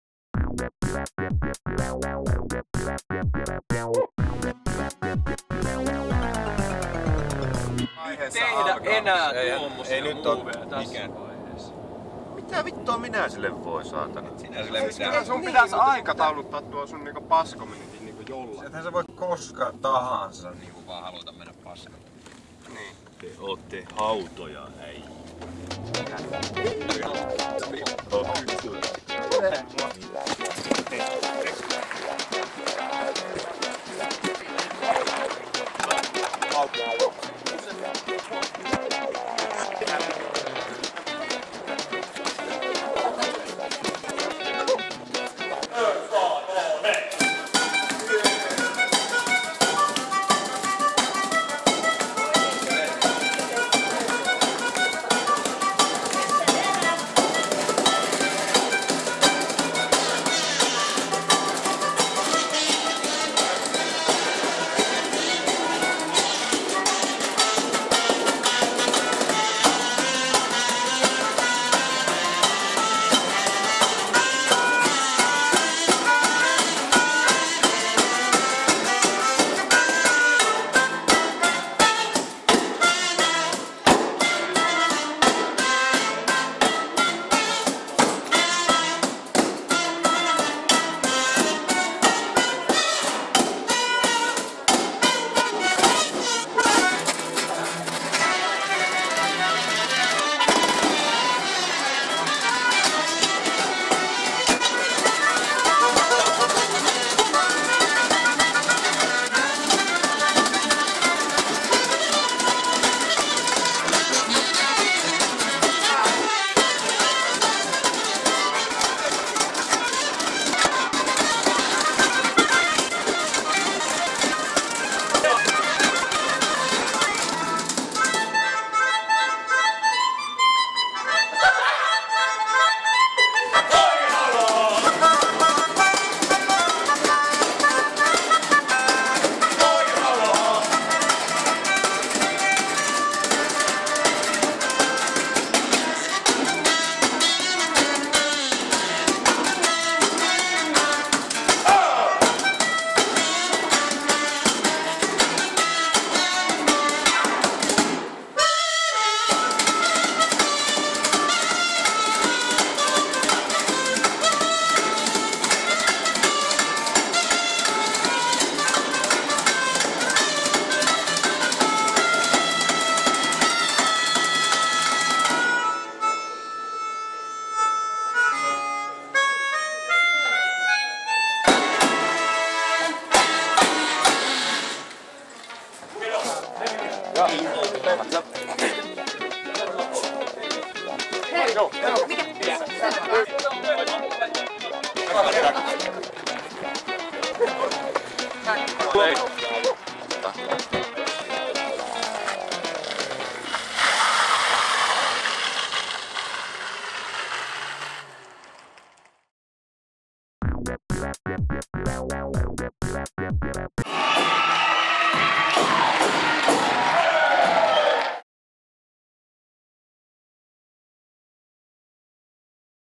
Ässä-TV / Jakso #1 - Pistokeikka
Neljä Ässää käy keikalla Neste Tuuliruusussa Mäntsälässä.

Neljä Ässää on:

Samuli Kivelä - Pata
Ville Aalto - Risti
Riku Nieminen - Ruutu
Aleksi Kinnunen - Hertta